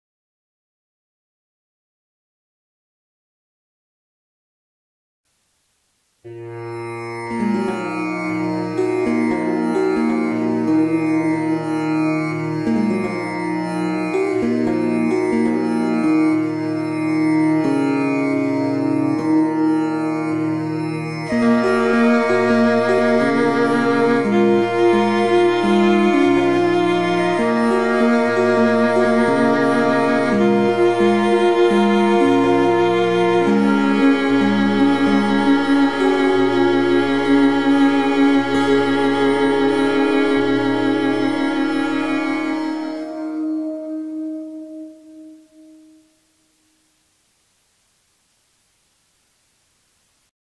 Shanti
Try out these calming songs, created using raag (raga) music theory.  Take the "chill pill" and chillax for a few minutes.  Peace...Shanti.

Instruments used:  choazhou,  shakuhachi